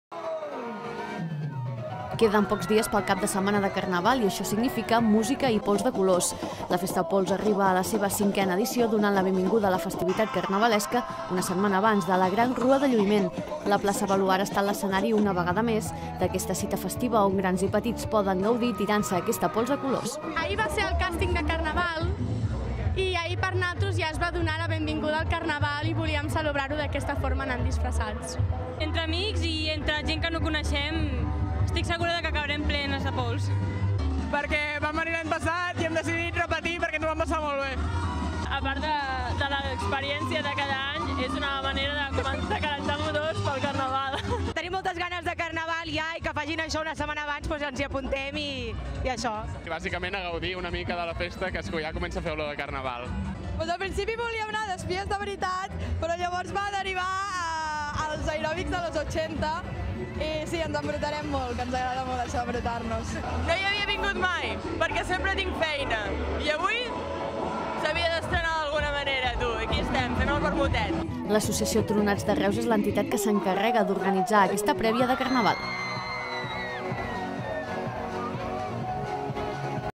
Fes-Te Pols dóna el tret de sortida al Carnaval
La Fes-te Pols ha deixat, un any més la plaça Baluard ple de color. Aquesta jornada prèvia de Carnestoltes ja s'ha consolidat al calendari carnavalesc en la seva 5a edició.